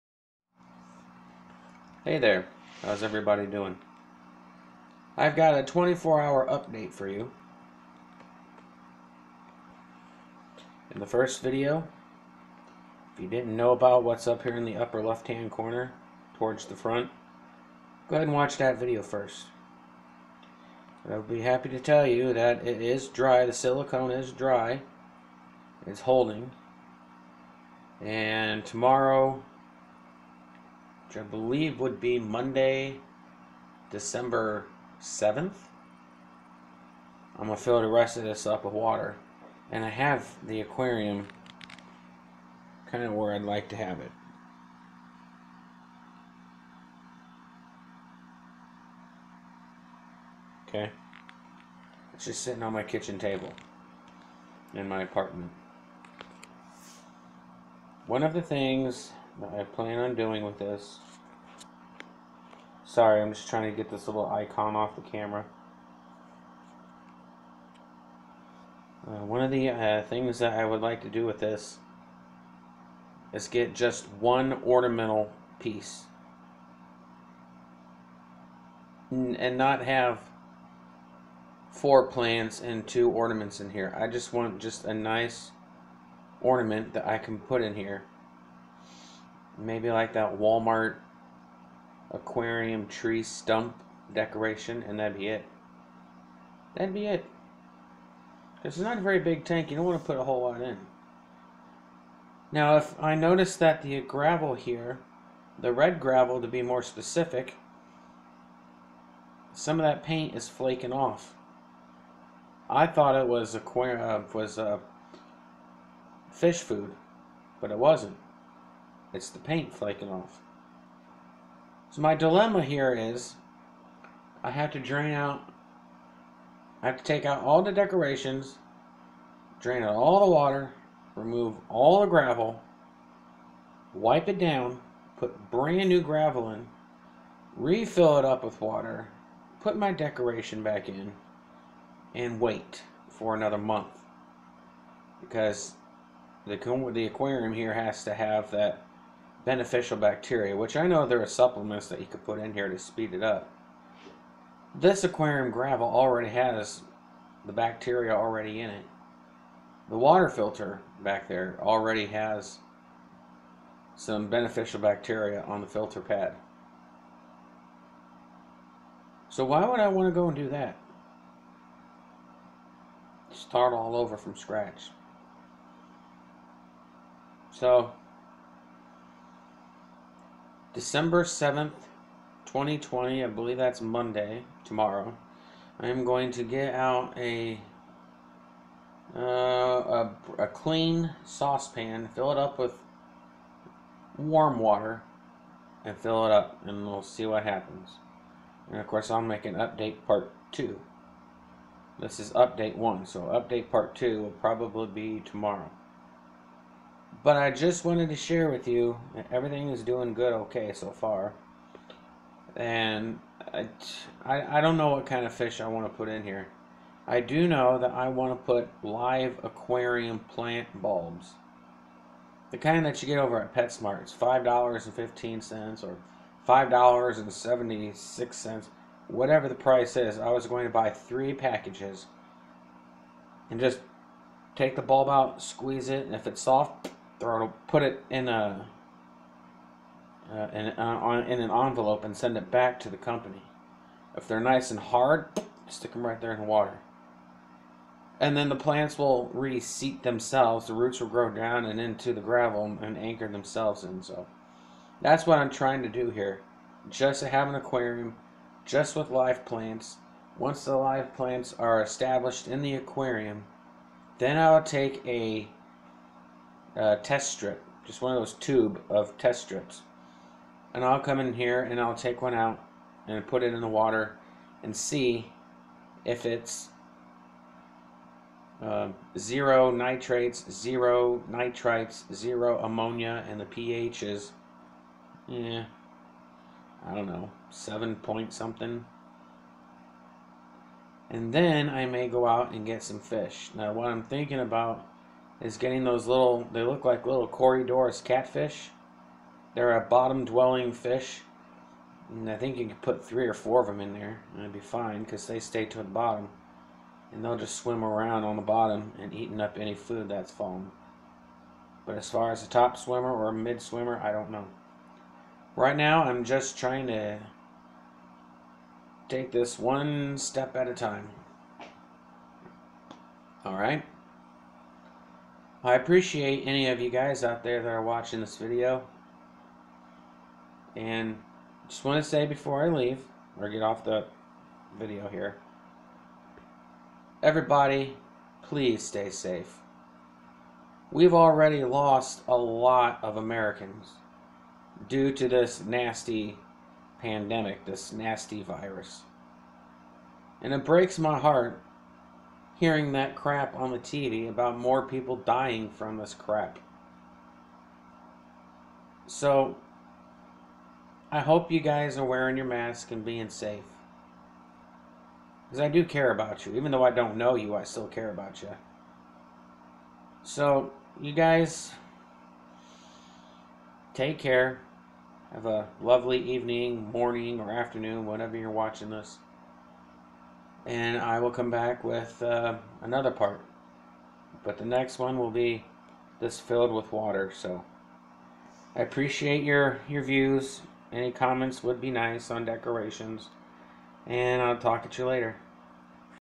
10 gallon aquarium update 2
The tank is holding water and no leaks. But the next video i will have taken out this decor and adding a heater and plant bulbs.